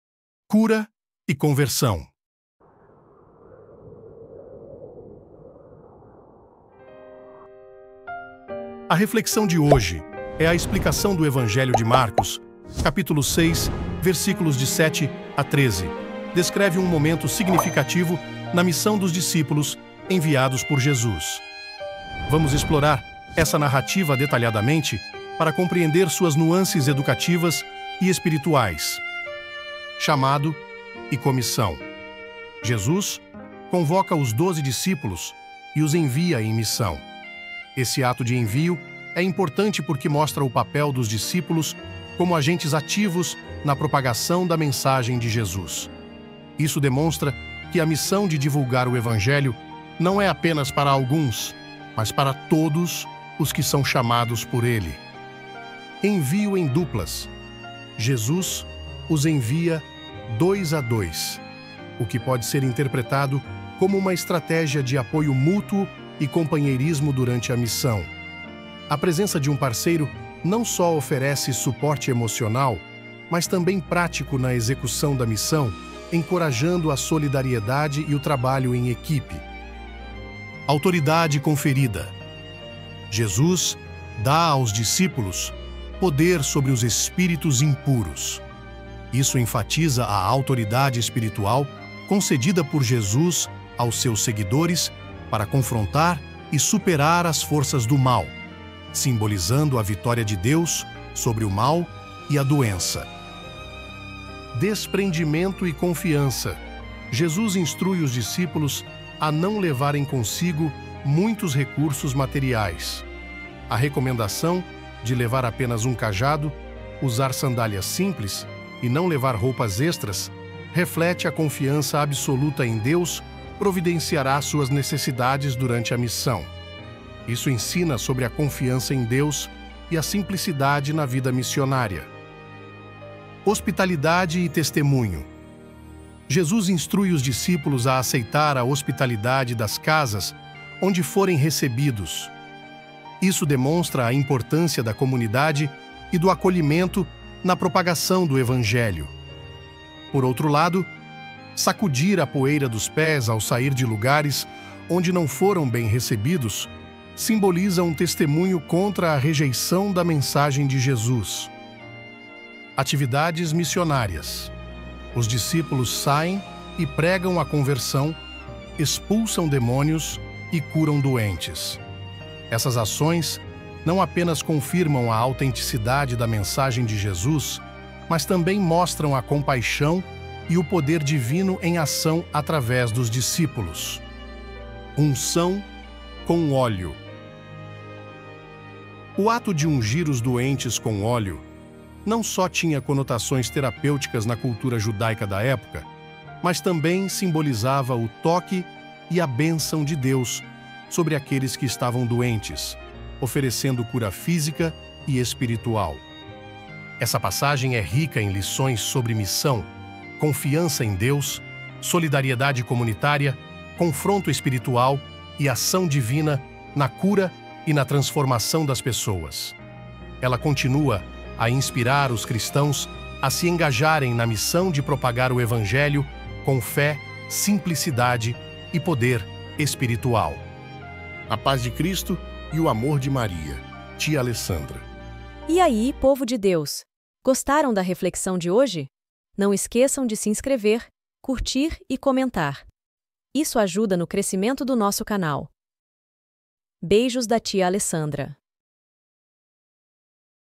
REFLEXÃO DE HOJE 14/07/2024 | Explicação do Evangelho | Cura e Conversão | Mc 6,7-13
Reflexão de Hoje – Explicação do Evangelho - Cura e Conversão.
A reflexão de Hoje é a explicação do Evangelho de Marcos capítulo 6, versículos de 7 a 13, descreve um momento significativo na missão dos discípulos enviados por Jesus. Vamos explorar essa narrativa detalhadamente para compreender suas nuances educativas e espirituais.
Chamado e Comissão: Jesus convoca os doze discípulos e os envia em missão. Esse ato de envio é importante porque mostra o papel dos discípulos como agentes ativos na propagação da mensagem de Jesus. Isso demonstra que a missão de divulgar o evangelho não é apenas para alguns, mas para todos os que são chamados por Ele.
Envio em Duplas: Jesus os envia "dois a dois", o que pode ser interpretado como uma estratégia de apoio mútuo e companheirismo durante a missão. A presença de um parceiro não só oferece suporte emocional, mas também prático na execução da missão, encorajando a solidariedade e o trabalho em equipe.
Autoridade Conferida: Jesus dá aos discípulos poder sobre os espíritos impuros. Isso enfatiza a autoridade espiritual concedida por Jesus aos seus seguidores para confrontar e superar as forças do mal, simbolizando a vitória de Deus sobre o mal e a doença.
Desprendimento e Confiança: Jesus instrui os discípulos a não levarem consigo muitos recursos materiais. A recomendação de levar apenas um cajado, usar sandálias simples e não levar roupas extras reflete a confiança absoluta em Deus providenciará suas necessidades durante a missão. Isso ensina sobre a confiança em Deus e a simplicidade na vida missionária.
Hospitalidade e Testemunho: Jesus instrui os discípulos a aceitar a hospitalidade das casas onde forem recebidos. Isso demonstra a importância da comunidade e do acolhimento na propagação do evangelho. Por outro lado, sacudir a poeira dos pés ao sair de lugares onde não foram bem recebidos simboliza um testemunho contra a rejeição da mensagem de Jesus.
Atividades Missionárias: Os discípulos saem e pregam a conversão, expulsam demônios e curam doentes. Essas ações não apenas confirmam a autenticidade da mensagem de Jesus, mas também mostram a compaixão e o poder divino em ação através dos discípulos.
Unção com Óleo: O ato de ungir os doentes com óleo não só tinha conotações terapêuticas na cultura judaica da época, mas também simbolizava o toque e a bênção de Deus sobre aqueles que estavam doentes, oferecendo cura física e espiritual.
Essa passagem é rica em lições sobre missão, confiança em Deus, solidariedade comunitária, confronto espiritual e ação divina na cura e na transformação das pessoas. Ela continua a inspirar os cristãos a se engajarem na missão de propagar o evangelho com fé, simplicidade e poder espiritual.
Tia Alessandra